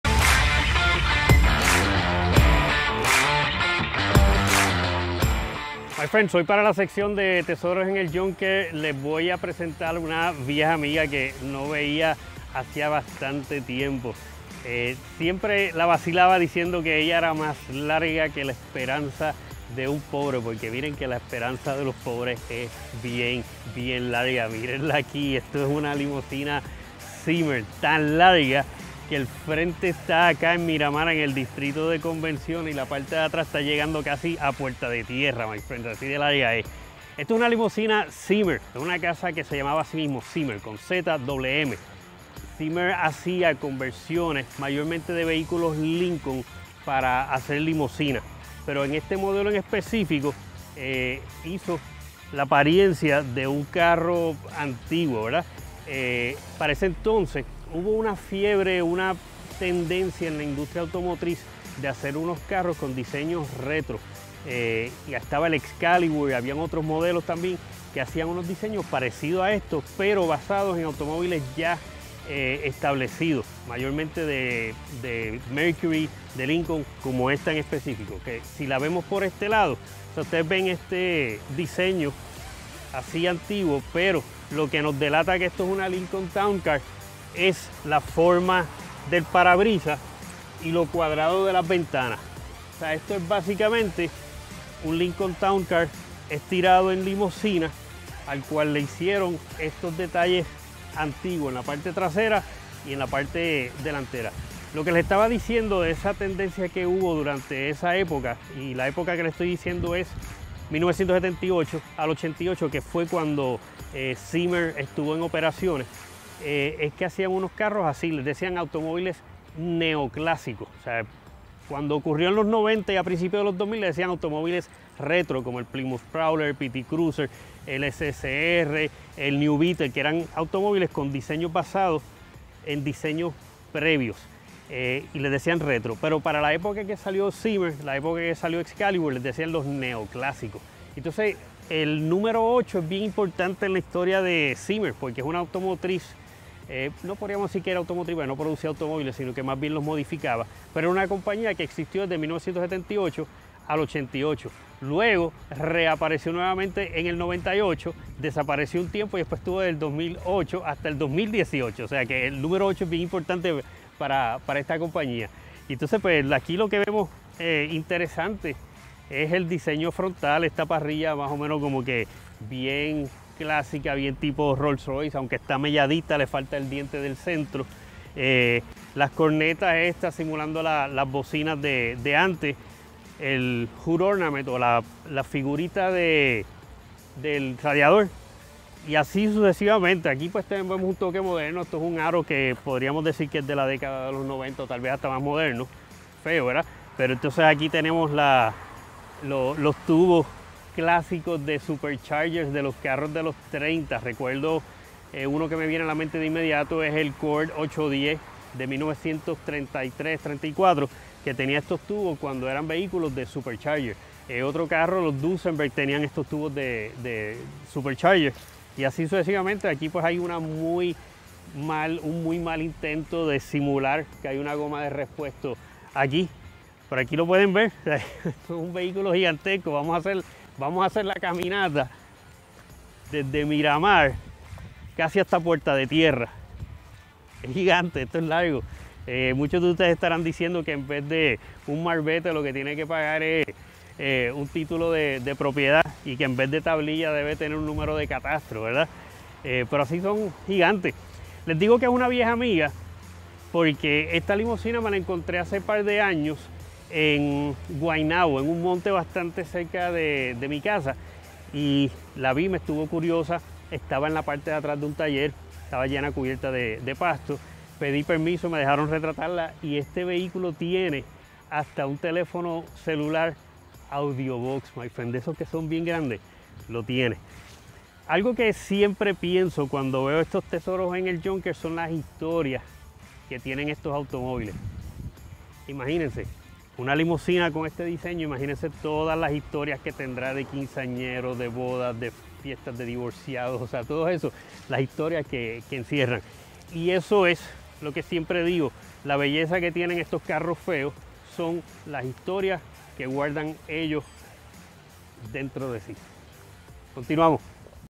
REENCUENTRO con una EXTRAÑA limusina ZIMMER
ANDRÉS O'NEILL se reencuentra en San Juan con la misma EXTRAÑA limusina ZIMMER que había encontrado hace años en un campo en Guaynabo. De paso, cuenta la INTERESANTE historia de la compañía Zimmer.